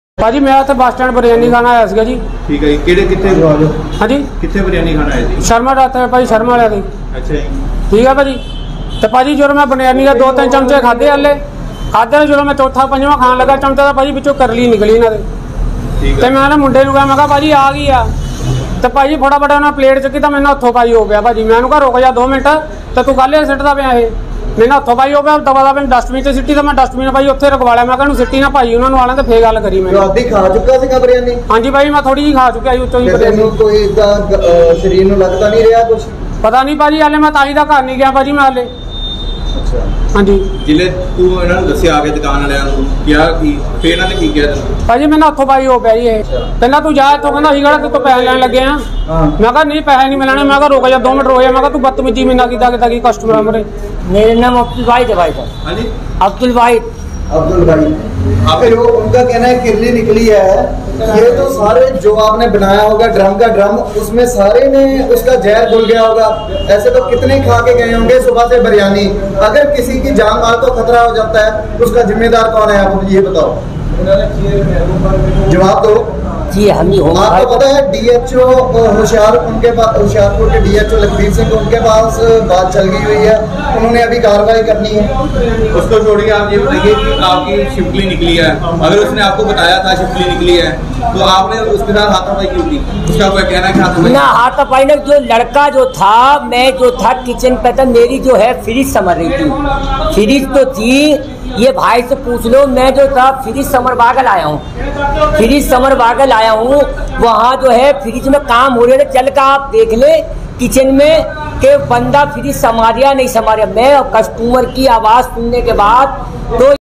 ਹੁਸ਼ਿਆਰਪੁਰ ਦੇ Bus stand ਨਜ਼ਦੀਕ ਇੱਕ ਢਾਬੇ ਤੋਂ ਬਰਿਆਨੀ ਚੋਂ ਨਿਕਲੀ ਕਿਰਲੀ
ਹੁਸ਼ਿਆਰਪੁਰ ਦੇ ਬੱਸ ਸਟੈਂਡ ਨਜ਼ਦੀਕ ਇੱਕ ਢਾਬੇ ਤੋਂ ਬਰਿਆਨੀ ਚੋਂ ਨਿਕਲੀ ਕਿਰਲੀ